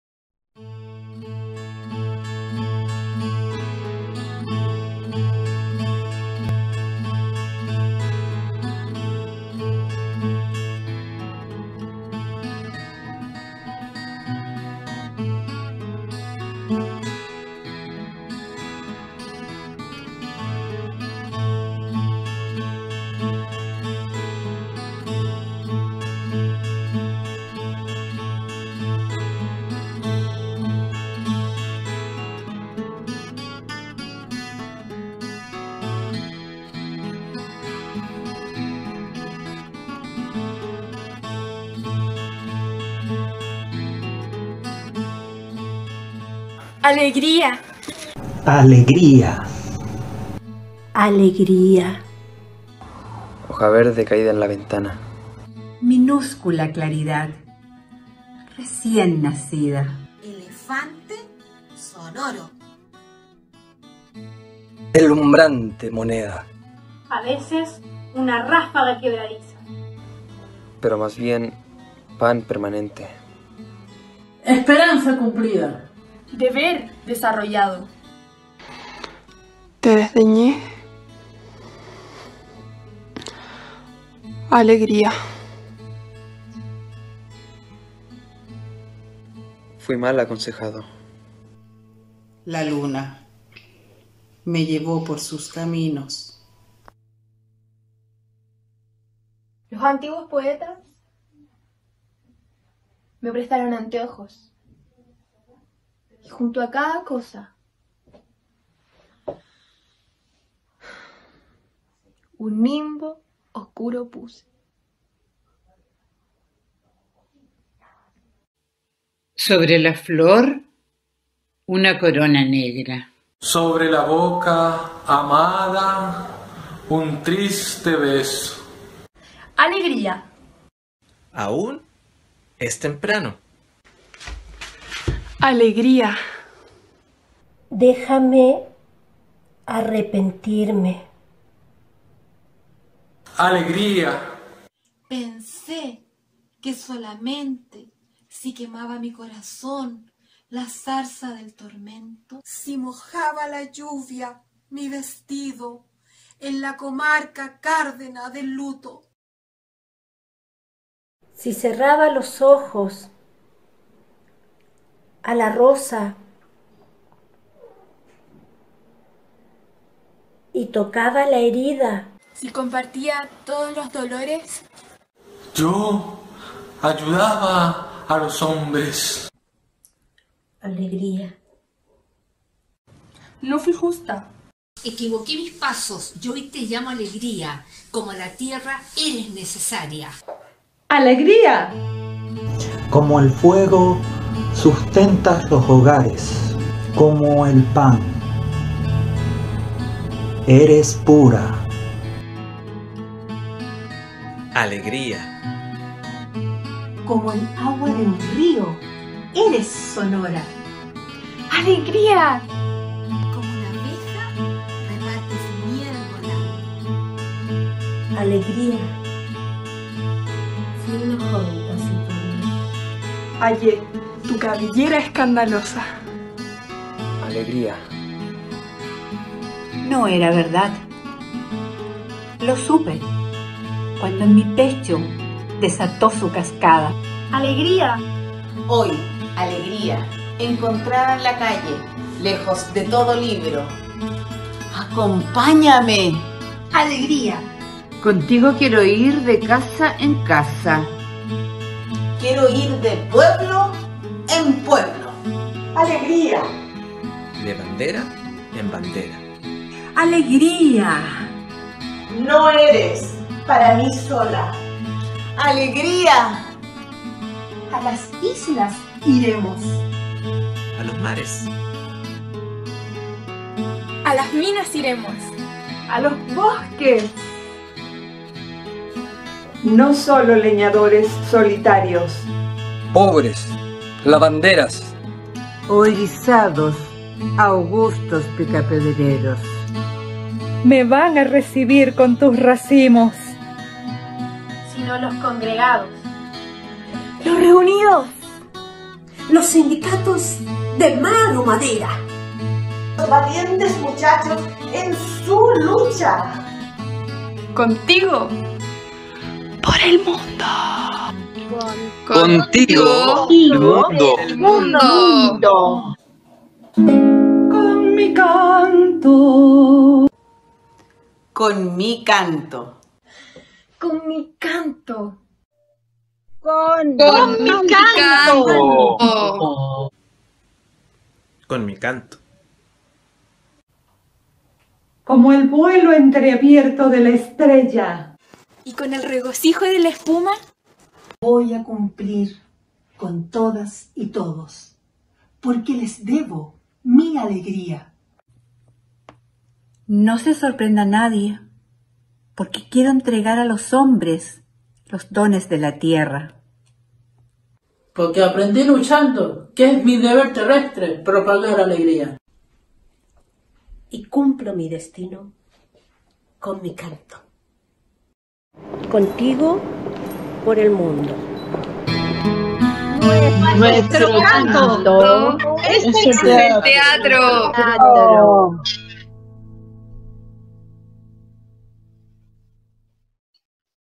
Oda "Alegría"- Trabajo Final Taller Online "El Arte de Actuar"
Corporación Cultural de La Reina

Oda “Alegría” de Pablo Neruda
Presentación
Taller online “El Arte de Actuar” de Rolando Valenzuela

Este año el destacado actor y director Rolando Valenzuela ofreció un taller de actuación gratuito online en La Reina ¡La verdad fue todo un éxito! Participaron alumnos y alumnas no sólo de nuestra comuna, sino también de regiones y del extranjero. Ahora llenos de entusiasmo, nos sorprenden con su trabajo final: una interpretación libre de la oda “Alegría” de Pablo Neruda con música de Gabriel Huentemil Ortega.

¡Aplauso cerrado para todos y todas!

Actores: Cecilia Hernández, Pompeyo Lepe, Josefa Suárez, Richard Silva, Erika Flores, Margarita Marambio, Jeannette Rosentreter, Susana Poblete, Franco Rojas, Eugenia Escorza, Sophie Caviedes, Antonella Giorgi, Natalia Valenzuela, Gabriel Huentemil, Francisca Correa, Alfred Borner, Carmen Gloria Zúñiga, María Barreto, Laura Alarcón, María Adriana Levin y Fabián Suárez.
Dirección: Rolando Valenzuela